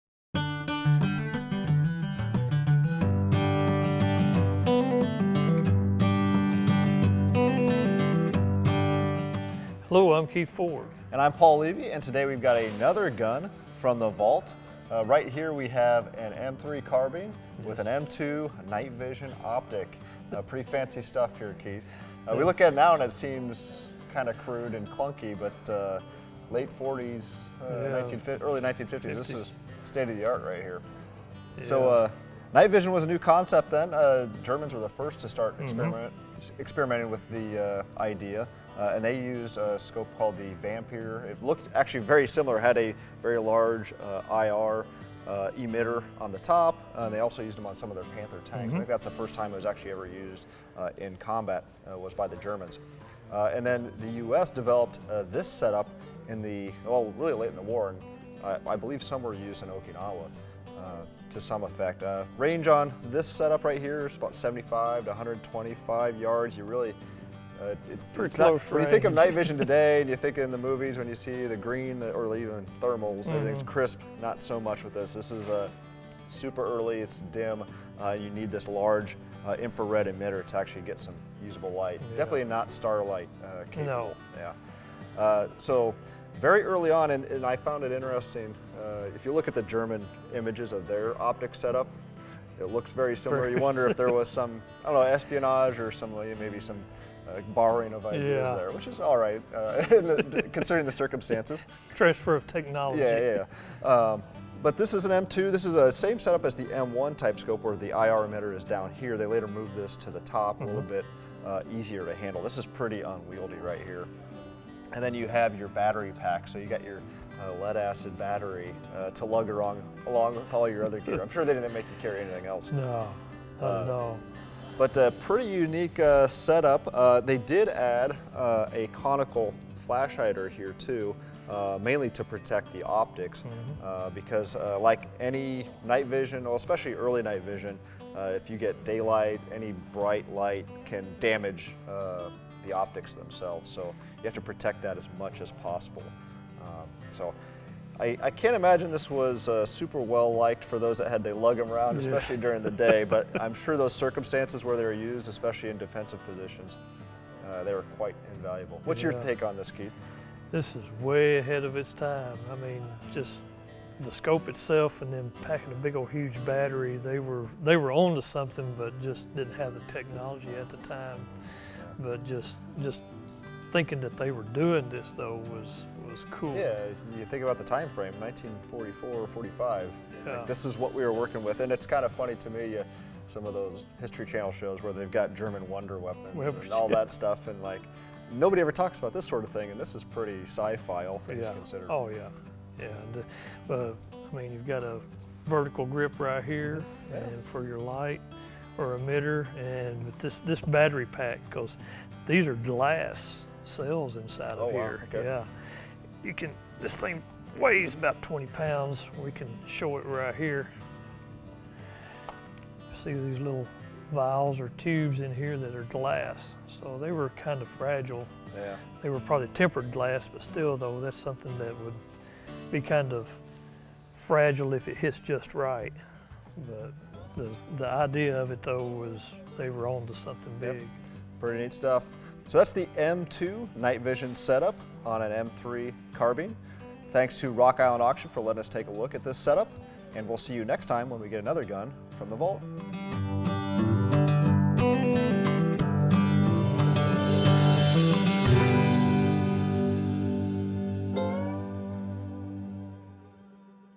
From the Vault: M3 Carbine with M2 Night Vision Sight
Brownells Gun Techs Keith Ford and Steve O... no wait, today it's new product guru Paul Levy, are at Rock Island Auction Co. to show us an M3 Carbine fitted with an M2 infrared night vision sight. Technically, the "M3" is an M2 Carbine fitted with the infrared sighting system; the M2 Carbine itself is the select-fire variant of the M1 Carbine. This is state of the art night vision technology for the mid- to late-1940s / Korean War Era. It has a large riflescope-like device on top of the Carbine, plus a huge IR emitter attached to the forend and an enormous 20 lb. lead acid battery pack (with GLASS cells inside) connected via a thick cable. Probably a better defensive weapon than something a soldier would carry on a long patrol! Range was 75 to 125 yards. A far cry from even a $200 night vision scope you can buy off the Internet today! It looks ungainly now, but 70 years ago the M2 infrared sighting system must've seemed like something out of science fiction.

A binge-watcher’s dream! View ALL our videos